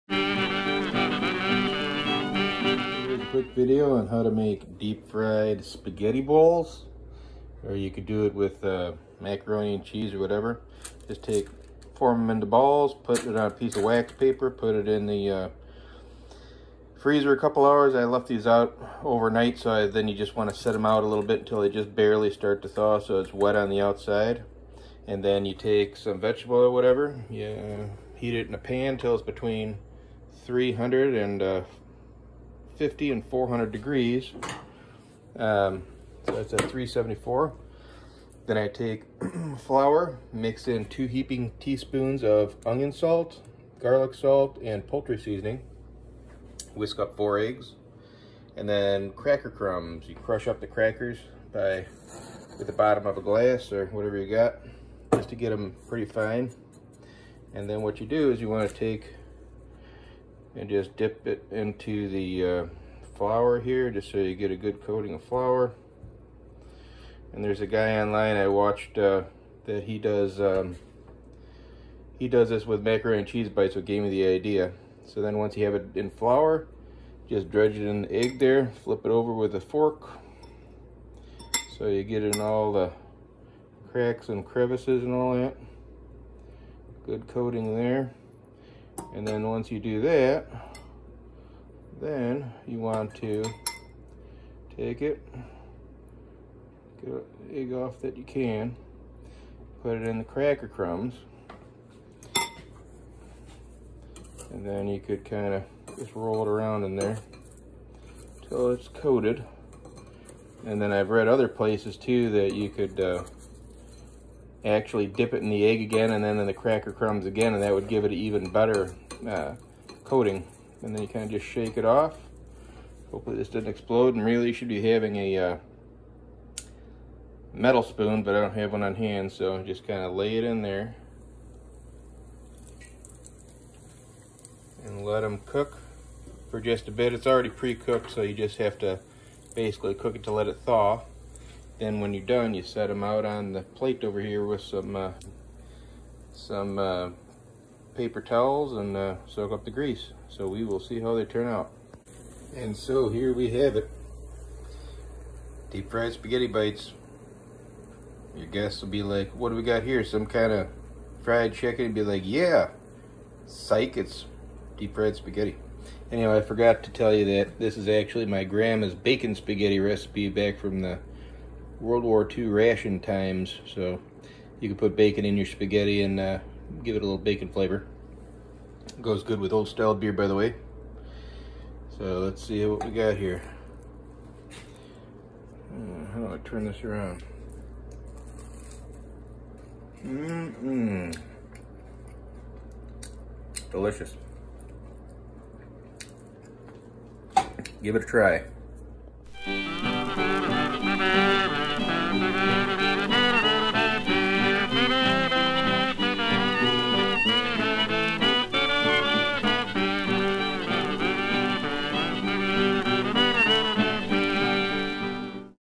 Deep Fried Spaghetti
If you have a hankering for spaghetti, but in a less healthy bite size wa give this a try…